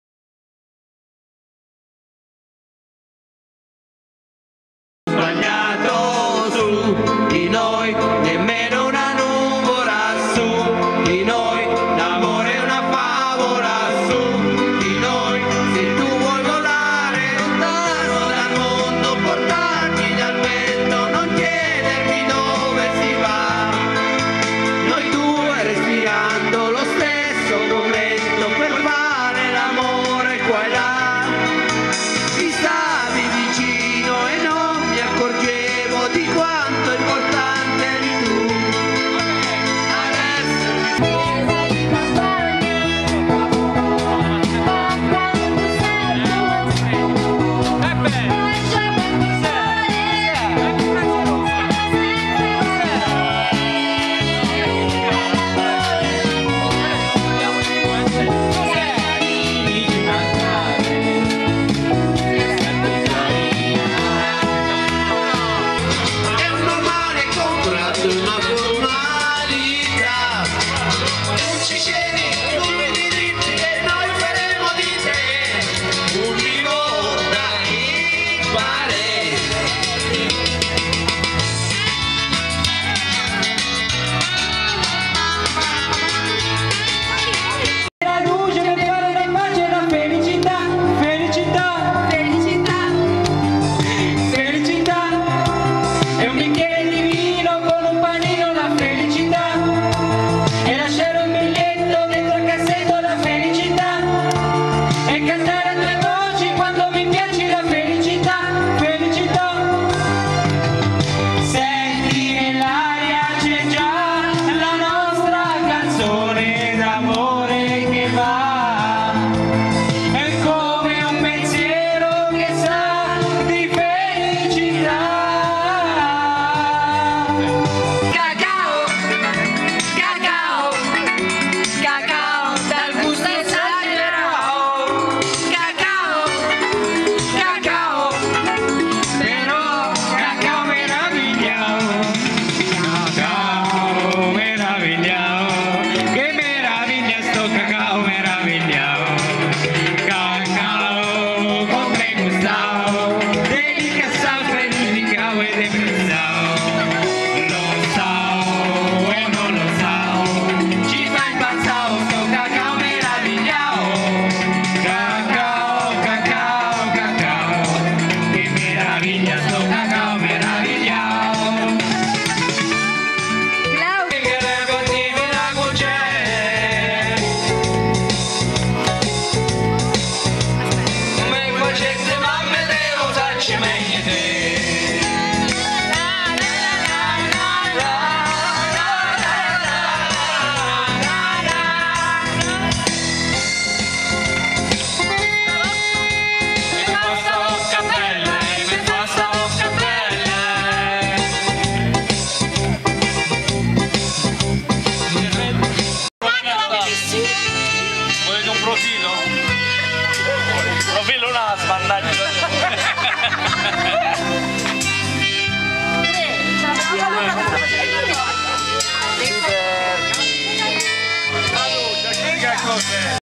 Cumpleaños 2008
Una noche en Italia con amigos festejando un cumpleaños